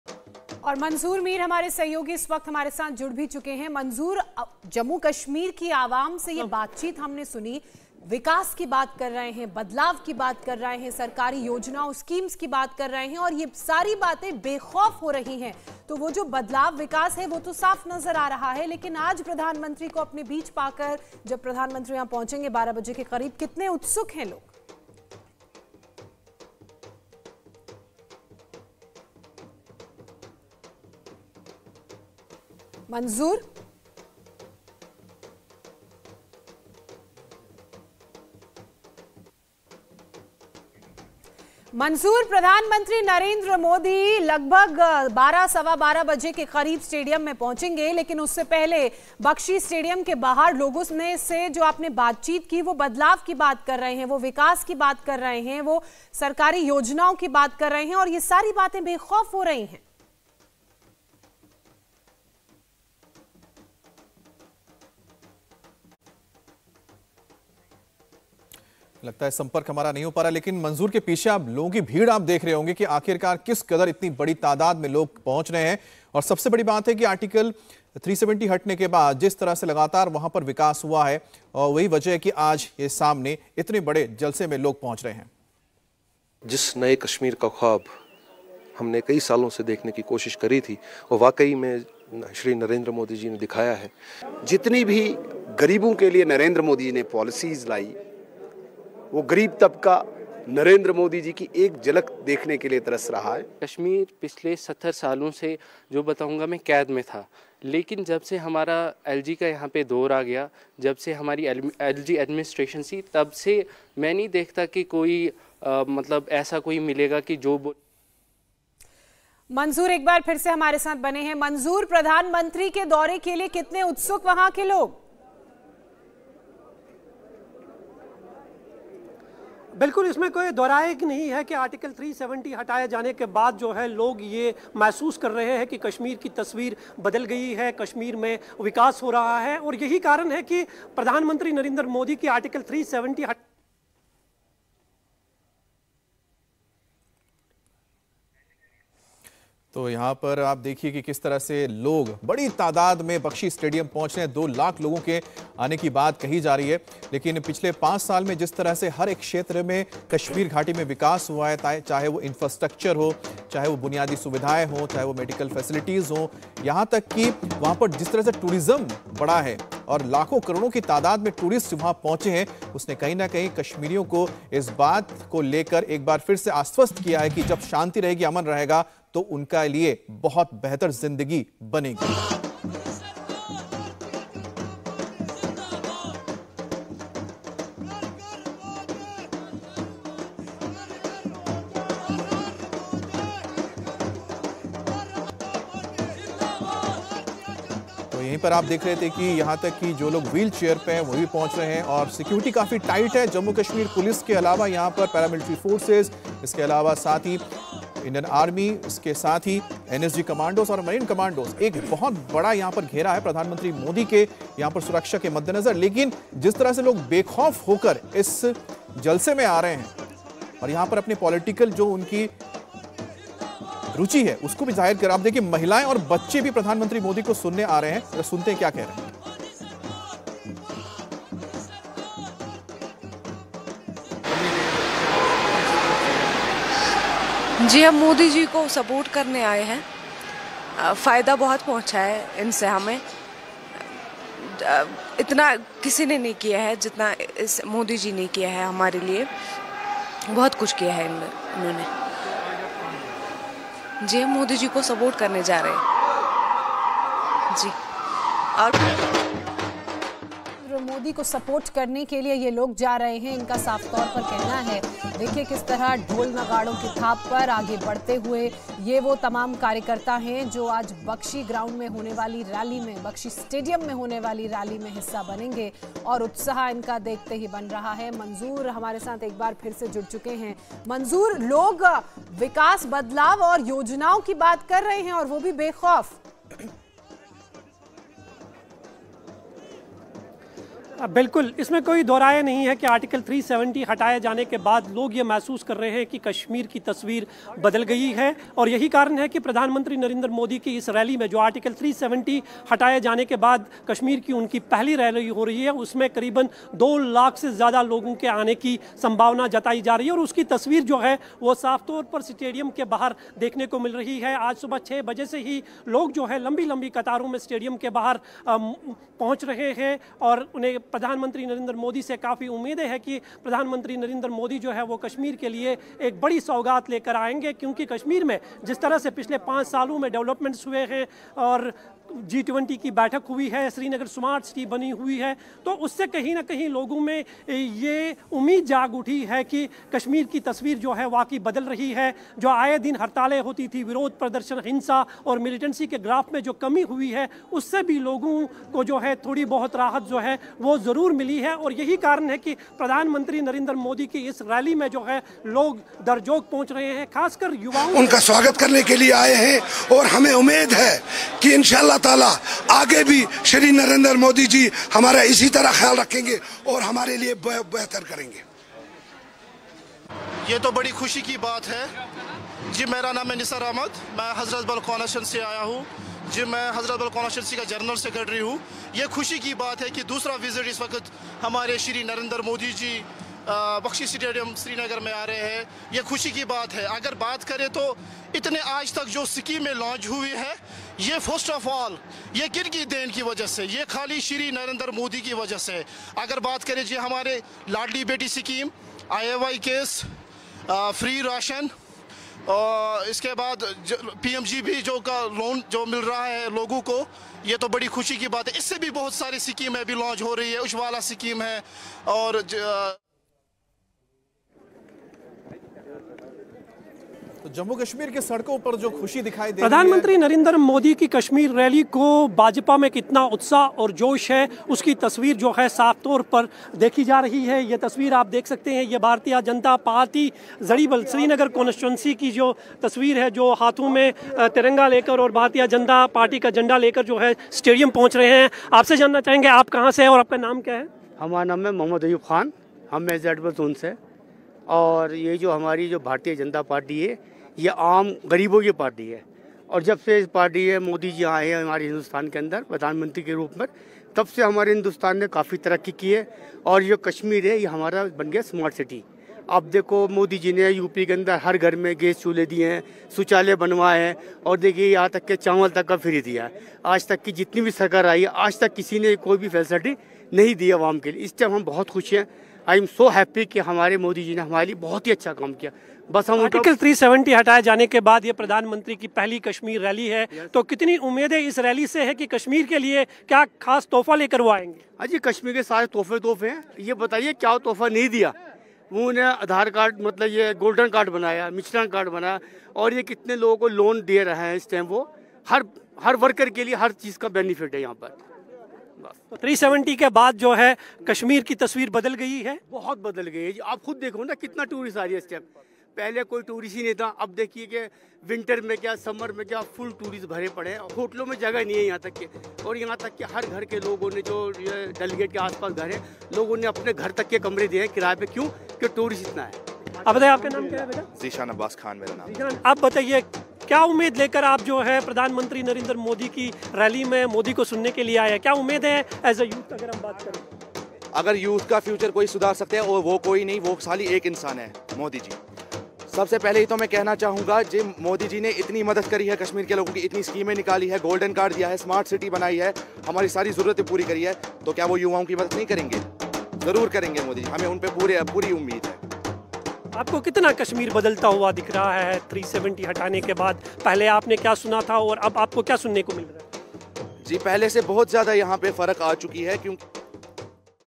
PM Modi In Kashmir: JK-आर्टिकल 370 हटने के बाद पीएम का श्रीनगर दौरा | Kashmir Tour | 2024
श्रीनगर में पीएम मोदी की रैली को लेकर सुरक्षा के पुख्ता इंतजाम किए गए हैं...जमीन से आसमान तक निगरानी की जा रही है... बख्शी स्टेडियम में पीएम मोदी को सुनने के लिए आज दो लाख से ज्यादा लोगों के पहुंचने का अनुमान हैं... सुबह से ही स्टेडियम के बाहर लोगों की 2 किलोमीटर लंबी लाइन दिख रही है...